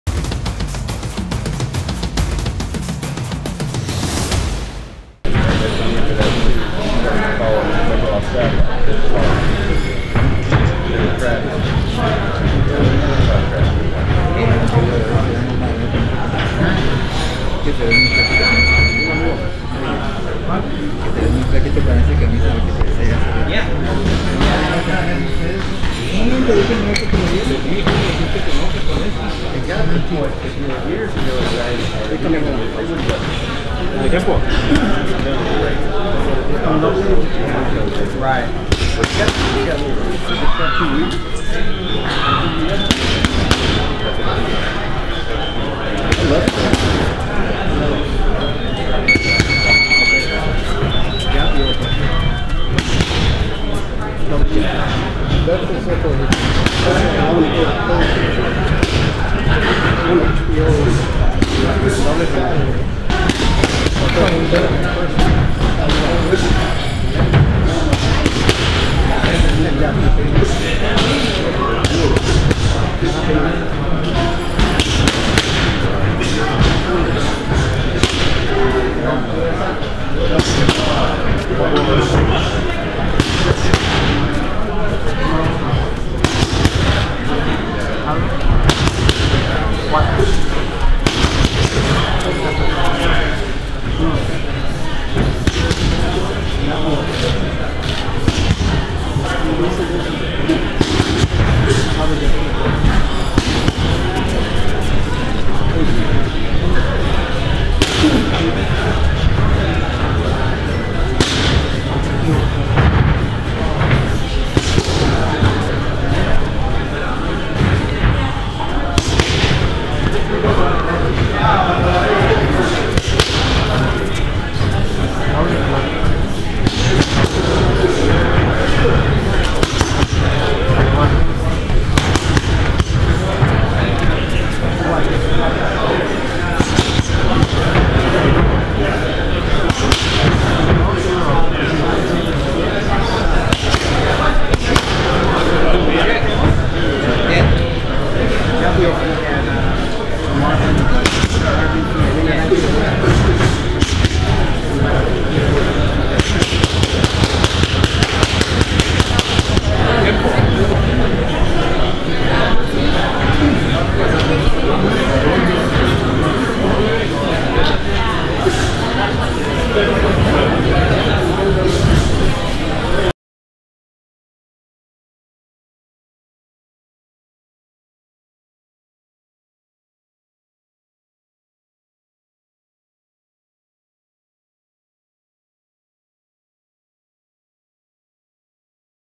Humberto Soto vs. Antonio Orozco full video-Complete Soto media workout video
In this Humberto Soto vs. Antonio Orozco full video, Humberto Soto holds his media workout as he prepares for his Soto vs. Orozco fight on HBO.